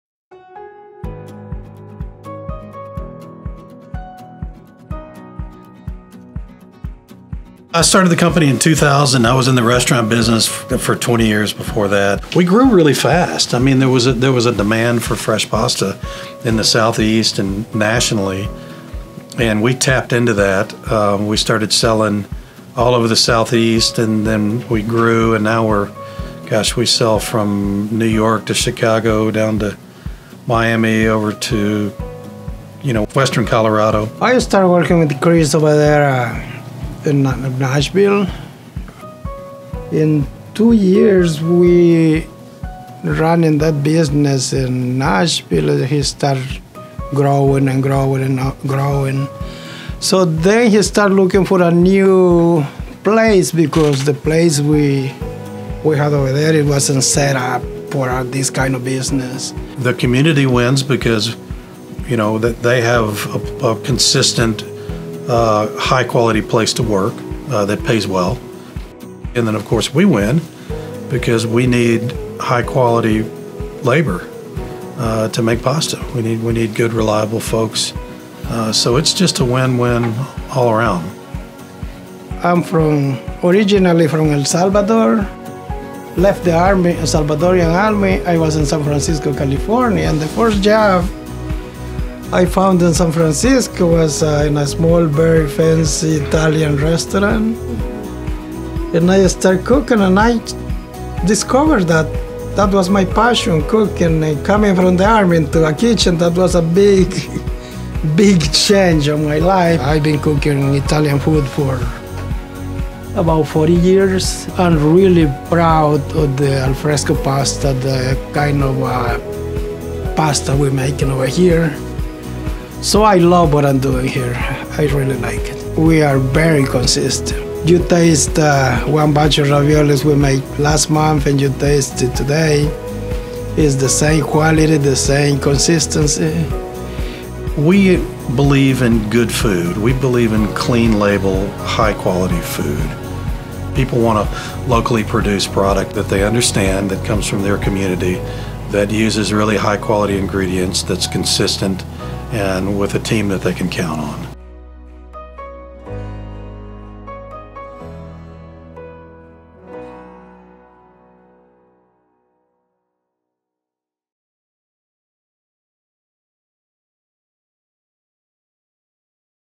From Music City to Rural Charm: Alfresco Pasta’s Move to Bells, Tennessee
In 2022, after more than 20 years of flourishing in Music City, Alfresco Pasta, a beloved producer of gourmet pasta, announced it would be relocating from Nashville to Bells, Tennessee, a town with a population of less than 2,500 people.

This strategic move was driven by the company’s need for more space to accommodate its growing operations, and in the last two years, it has brought significant benefits to both the company and local workforce.